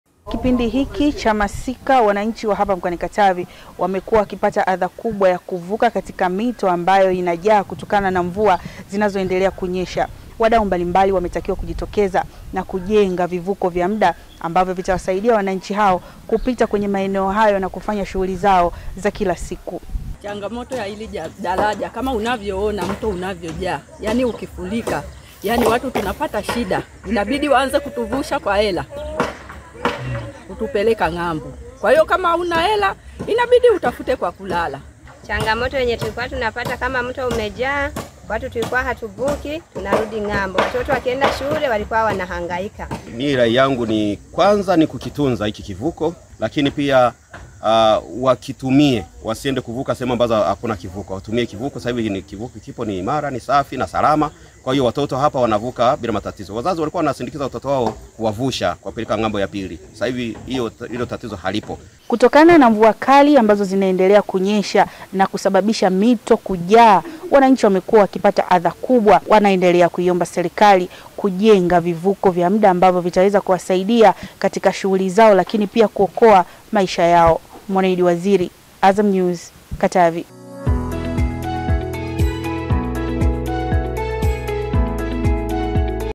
Wakazi Mpanda walilia daraja la ‘zege ya saruji na nondo’ badala ya mbao na miti
Wakazi wa wilaya ya Mpanda mkoani Katavi wameiomba serikali kuwajengea daraja la kudumu ili kuwaondolea adha na kuwaepusha na hatari ya kusombwa na maji hususani wakati wa kipindi cha mvua kubwa kutokana na daraja la mbao wanalotumia hivi sasa kukosa uimara wa kukabiliana na mvua kubwa.

Imeandaliwa na Mwanaidi Waziri || Wahariri @moseskwindi na Tuya Ibrahim